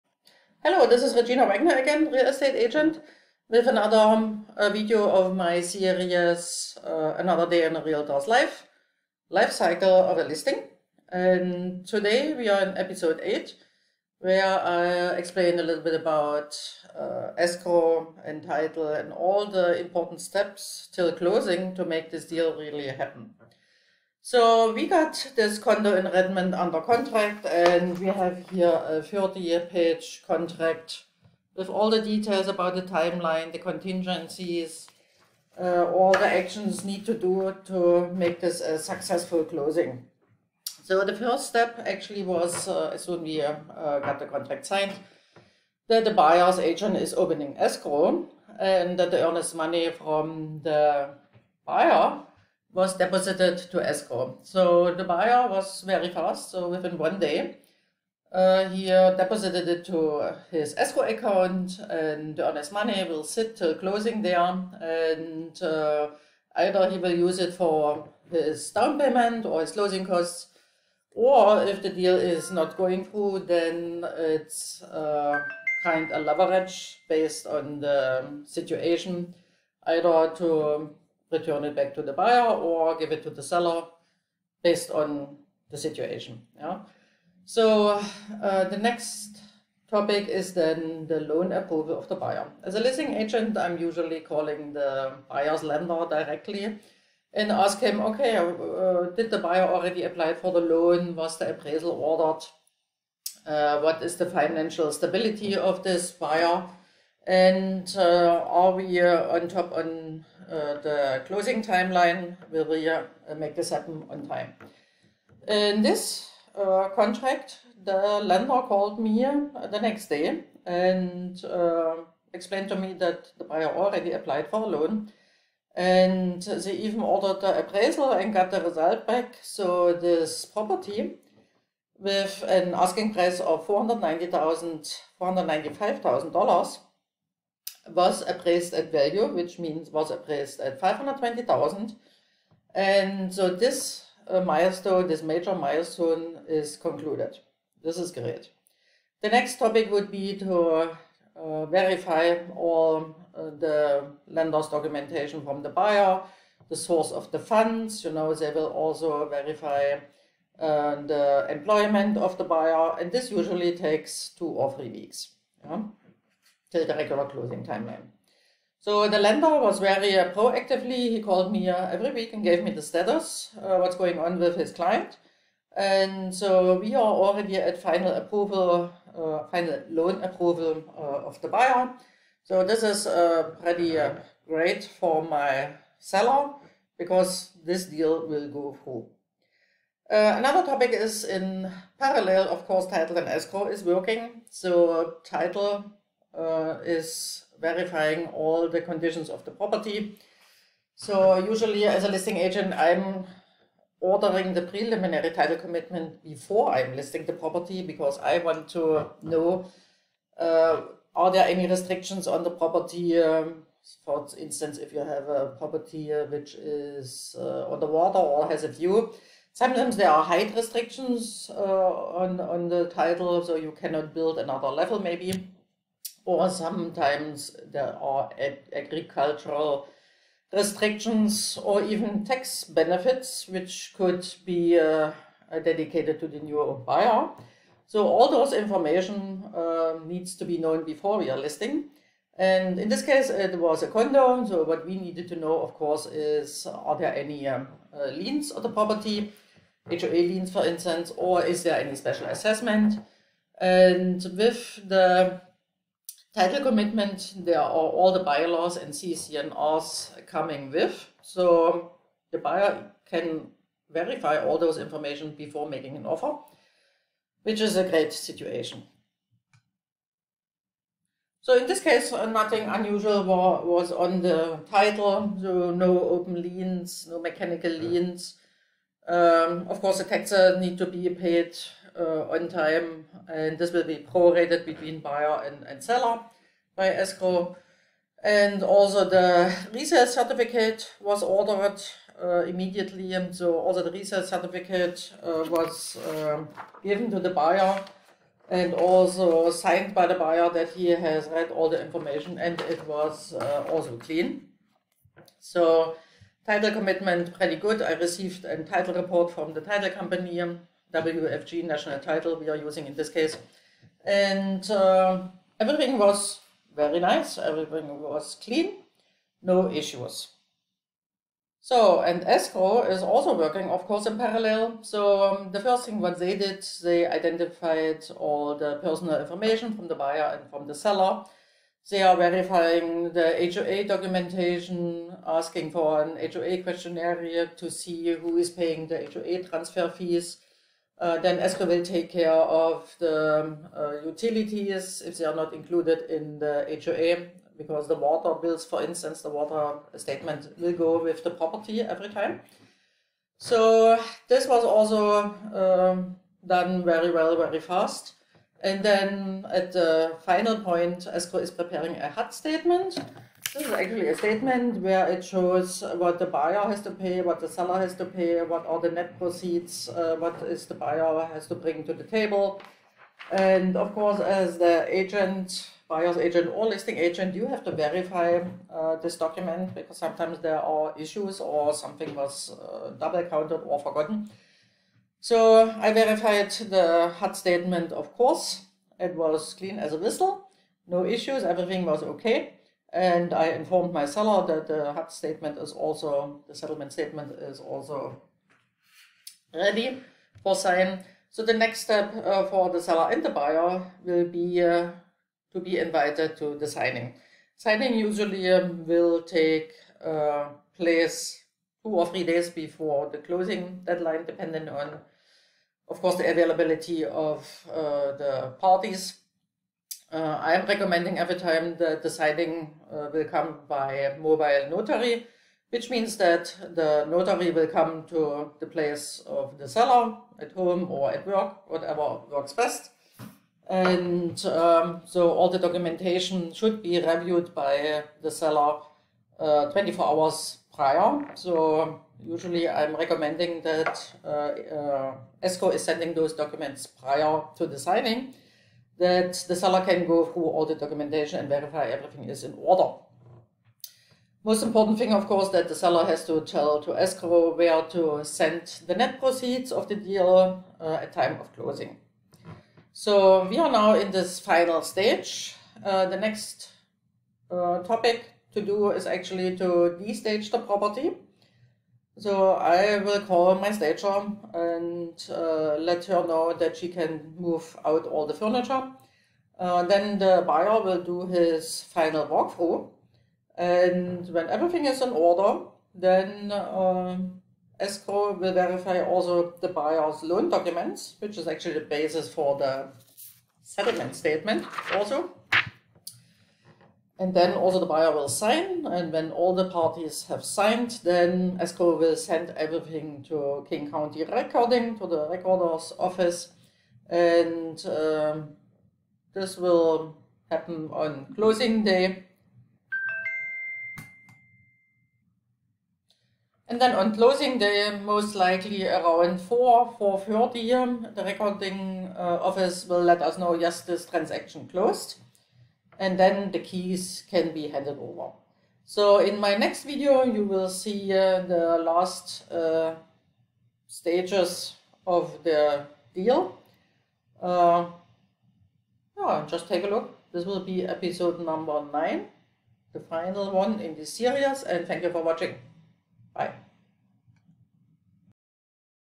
A day in a Realtors life - Listing life cycle -Episode 8 - UNDER CONTRACT
Redmond condo under contract, next steps for buyers lender, Escrow and Title company.